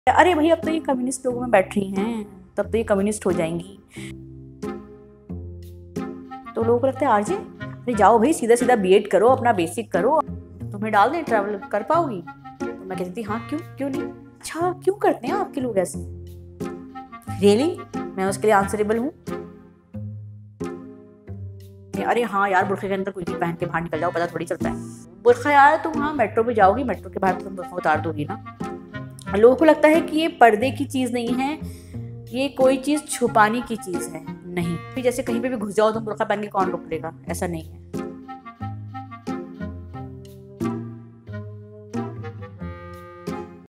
What Burkha is not!
What Muslim Women are Not. What the Burkha is Not.

One of the first Muslim female dastango, Fouzia Dastango gives us a funnier perspective on the general air around a Muslim woman in India.

Her quirky take on the stereotypes that surround the ‘made-up identity’ of Muslim women is hilarious and right on point!

Watch out this space for more breakthroughs on the IndianMuslimWoman – we are not ‘us’ and ‘them’, we are same.